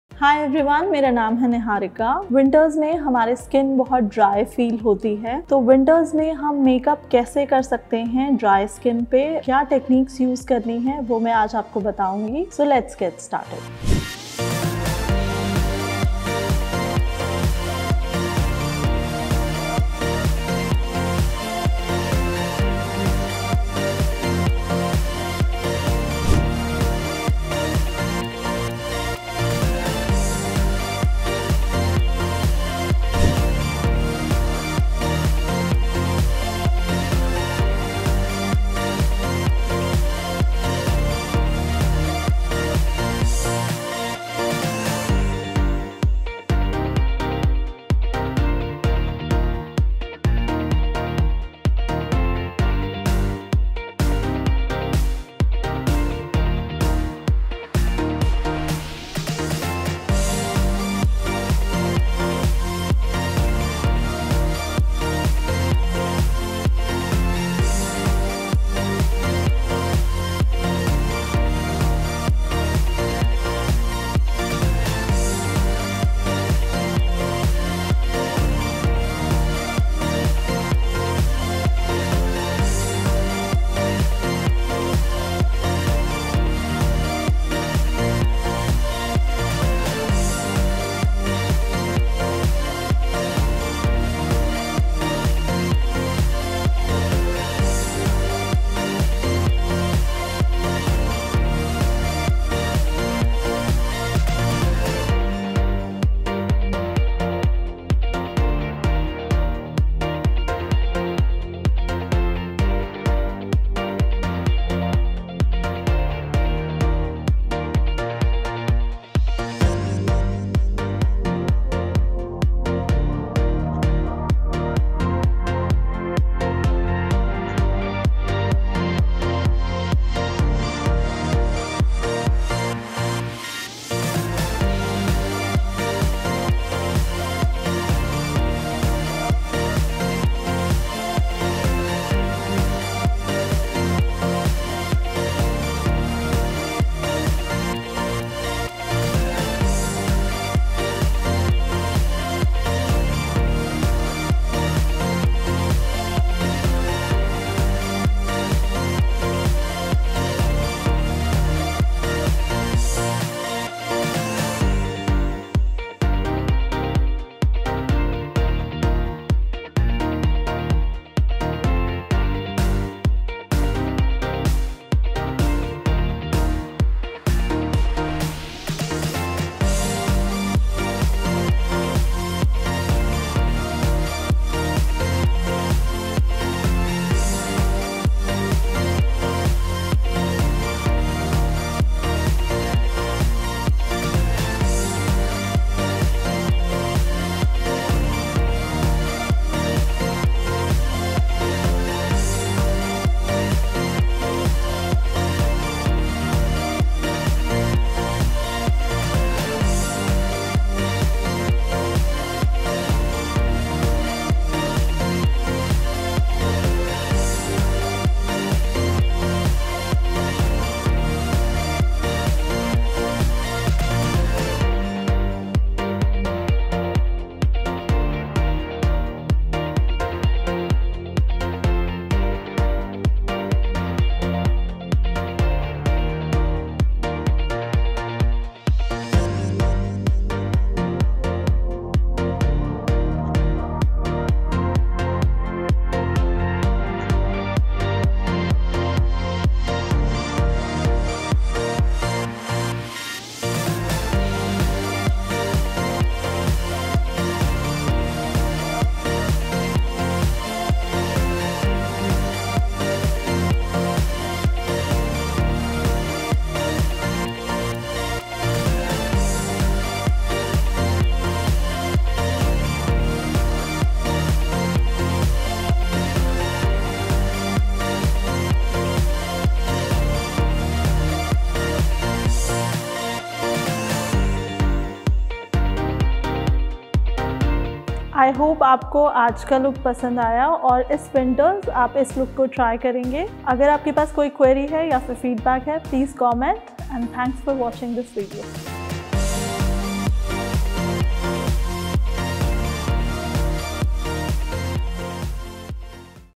Winter Makeup Look for Dry and Flaky Skin
Winter skin is generally dry and flaky which is why many of you may think twice before applying any makeup. FYI, you can still put on makeup and not get patchy skin. Here's a winter makeup tutorial for you if you love makeup!

Makeup Artist- Niharika Singh